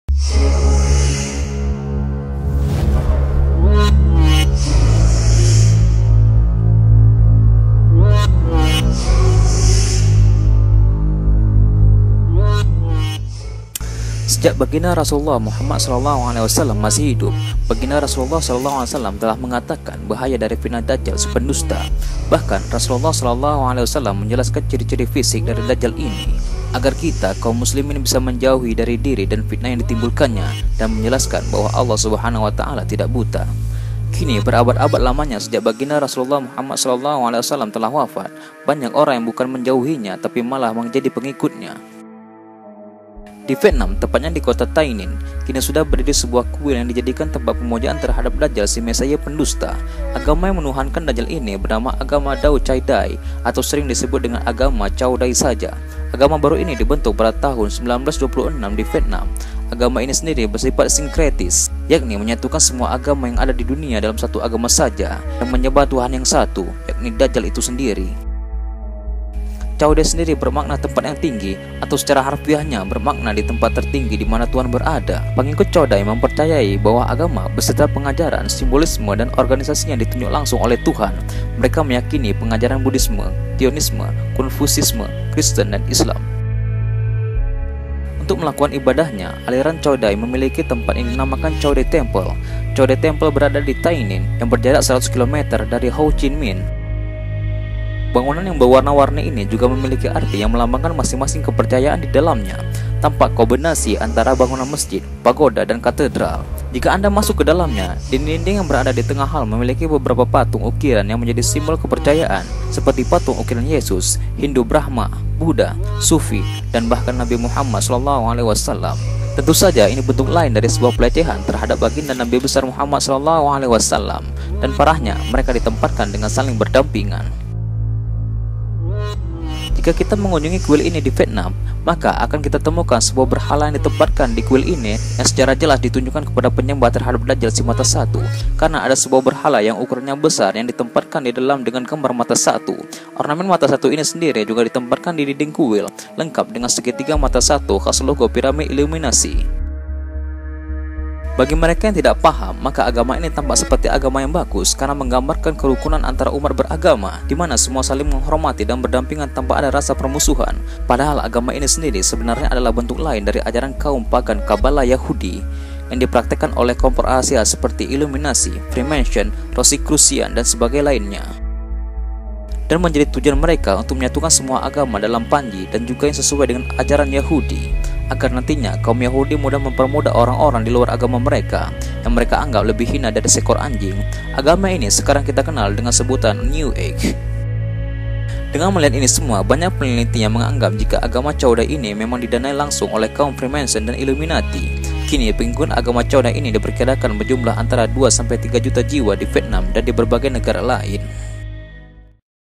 ADA PATUNG NABI ?? Inilah Agama Baru Penyembah DAJJAL Buatan Freemanson dan Iluminati
Cao dai adalah sebuah agama baru yang bersifat sinkretis yang dibentuk di Tây Ninh, Vietnam, pada tahun 1926. Cao Đài berarti tempat yang tinggi atau secara harfiahnya di tempat tertinggi di mana Tuhan berada